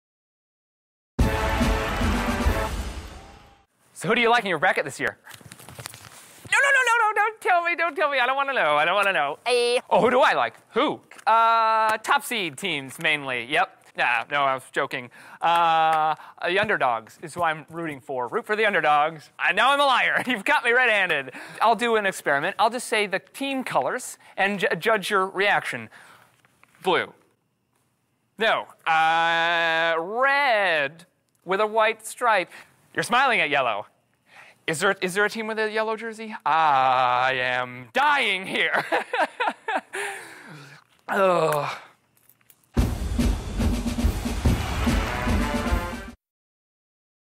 ESPN Bracketologists - The Fisherman
He lacks the background to make his own picks.  He wants to know what you know, but can't bring himself to ask.  His bracket is a patchwork of what he can gather from others. Will you see through his ruse or get caught in his net?